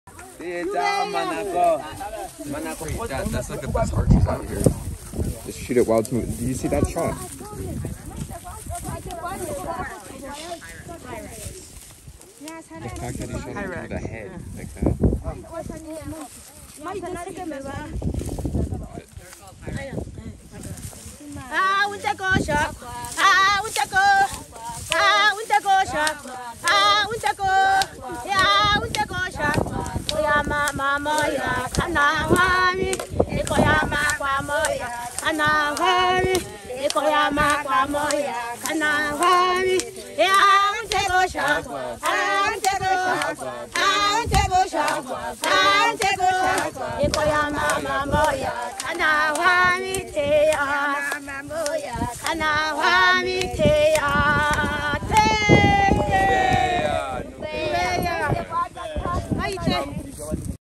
Hadzabe in Tanzania with the bush babies and dance
After killing two bush babies and a ferret (?), the woman in charge does a dance/chant.